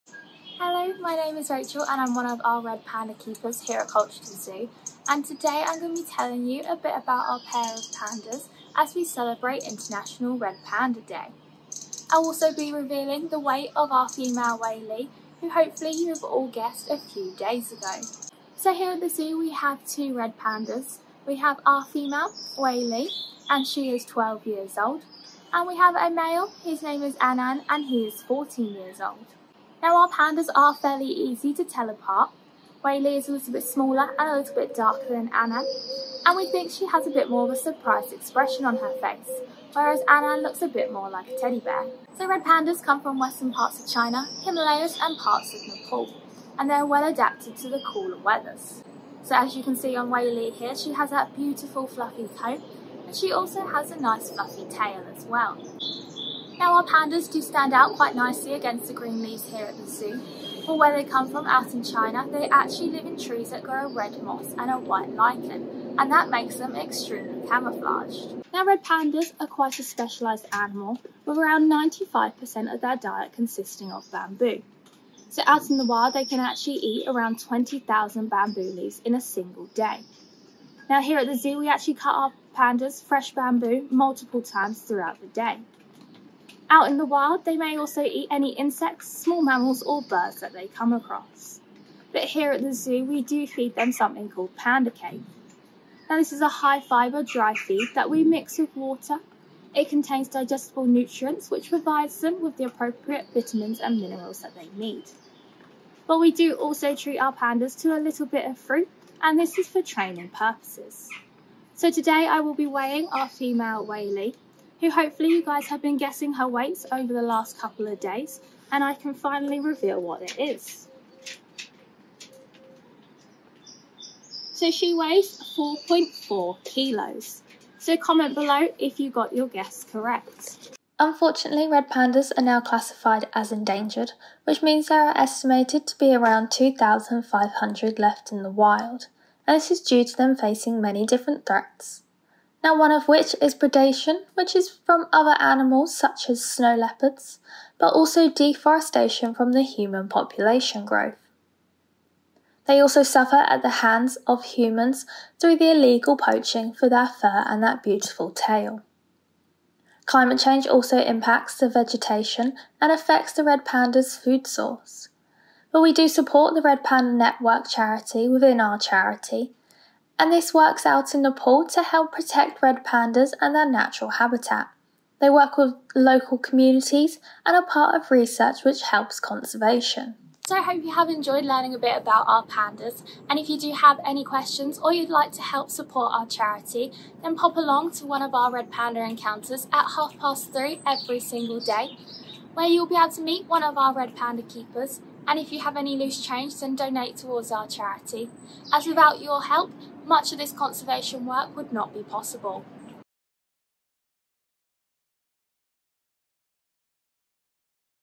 International Red Panda Day
What is small, red, fluffy, and incredibly cute? A Red Panda!
In celebration of International Red Panda Day, Zoo Keeper Rachel, introduces our pair, Liwei and An-An, and shares some interesting facts about this adorable species.